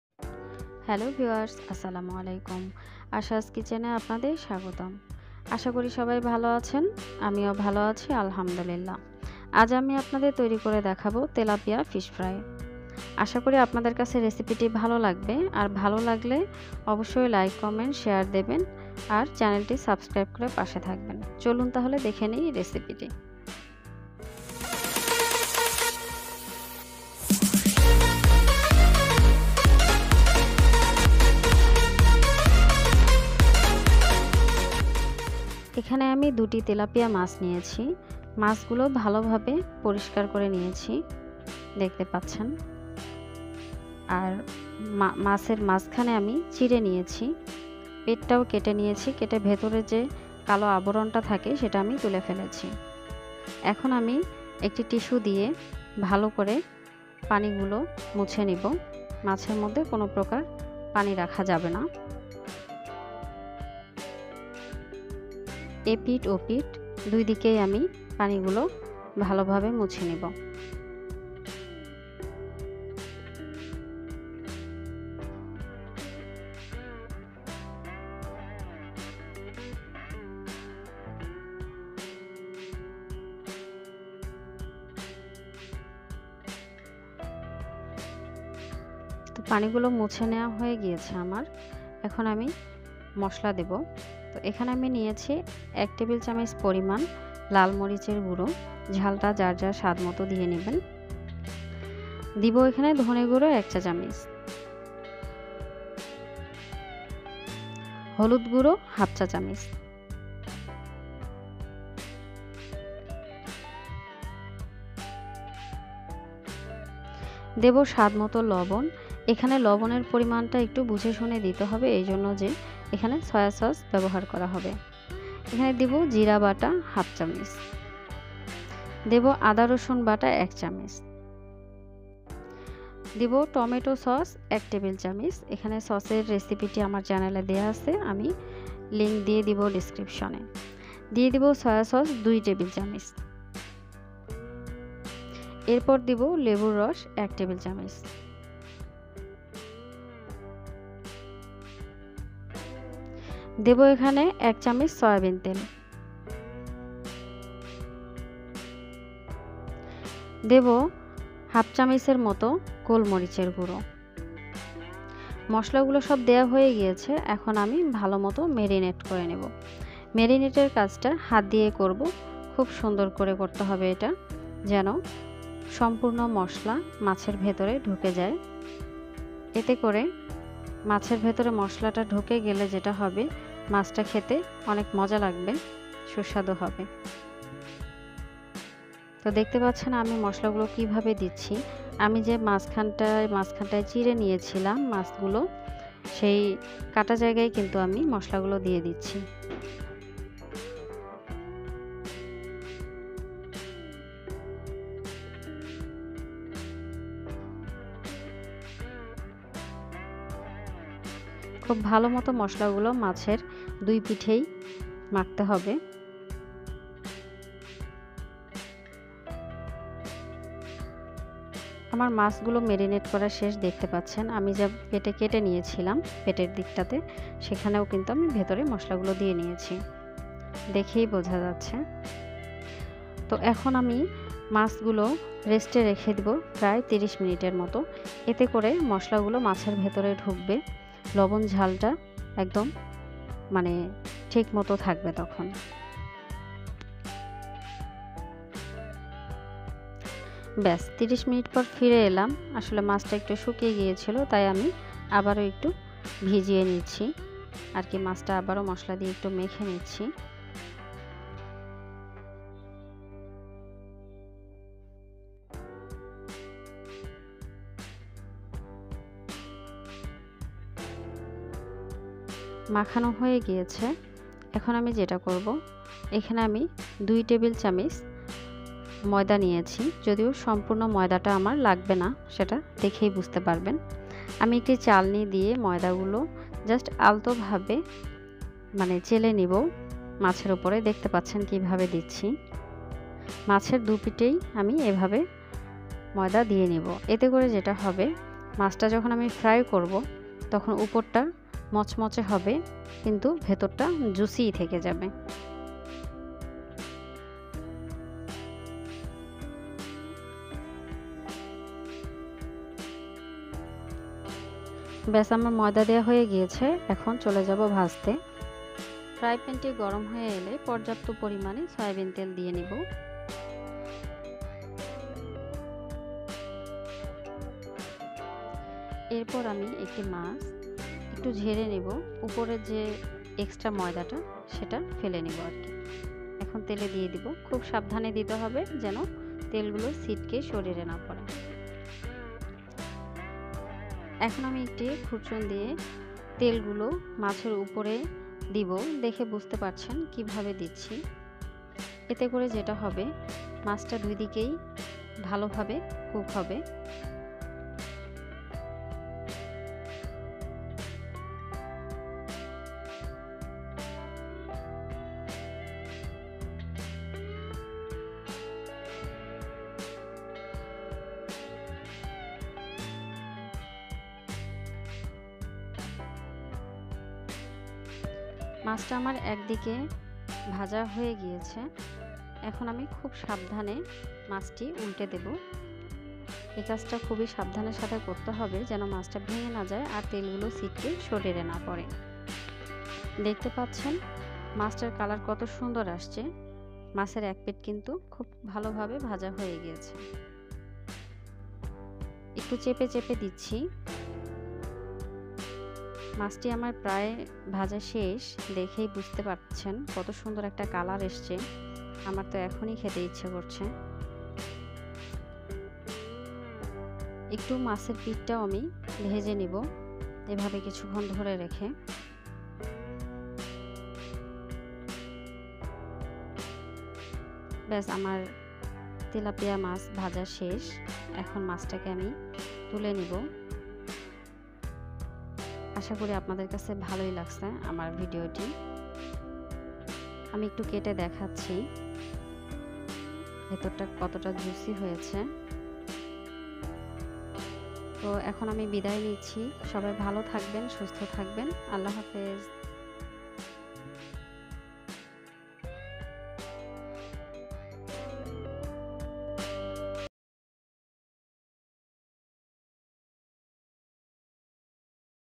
চমৎকার স্বাদের মচমচে তেলাপিয়া মাছ ভাজা || Tilapia fish fry || tilapia fish recipe
তেলাপিয়া ফিস ফ্রাই রেসিপি

উপকরণ :
তেলাপিয়া মাছ - ২টি
আদা-রসুন বাটা- ১ চা চামুচ
লাল মরিচ গুঁড়ো- ১ টে চামুচ
হলুদ গুড়ো - ১/২ চামুচ
ধনিয়া গুঁড়ো - ১ চামুচ
জিরা বাটা- ১ চামুচ
লেবুর রস- ১ টে: চামুচ
লবন- স্বাদমতো
সয়াসস- ২ টে : চামুচ
টমেটো সস- ১ টে: চামুচ
গোল মরিচ গুঁড়ো- ১/২ চামুচ
ময়দা- প্রয়োজন মত
সয়াবিন তেল- ভাজার জন্য